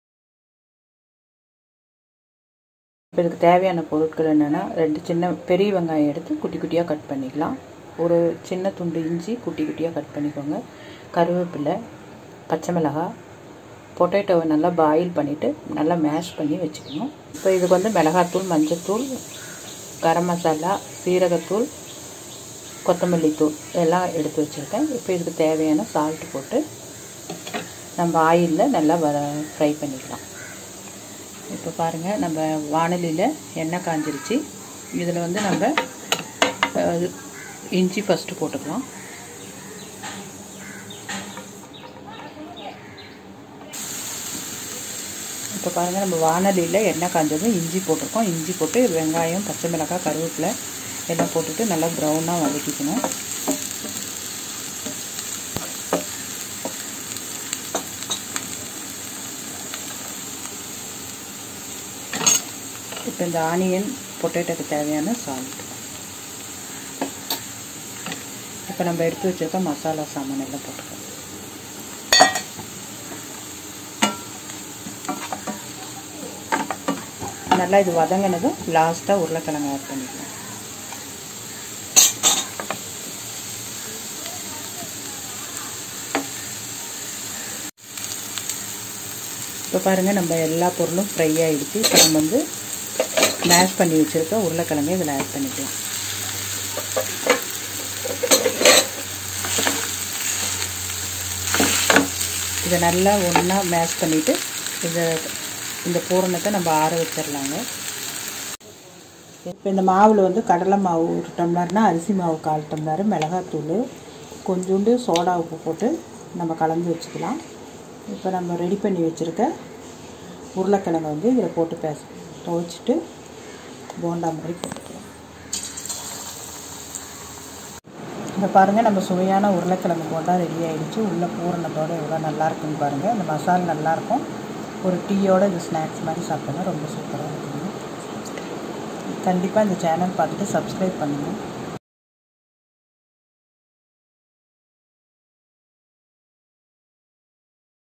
Masal Bonda Evening Snacks Part-2 Recipe
Masal Bonda Stuffing
Ingredients🛒
1.Oil
2.Onion
3.Smashed potato
4.Green chilli
5.Chopped ginger
6.Curry leaf
7.Coriander powder
8.Turmeric powder
9.Chilli powder
10.Garam masala
11.Cumin powder
12.Salt

Masal Bonda Batter
Ingredients🛒
1.Gram flour
2.Water
3.Chilli powder
4.Salt